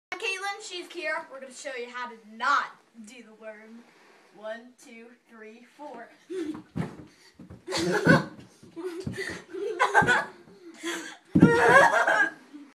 Not the Worm
Twins show how not to do the worm;)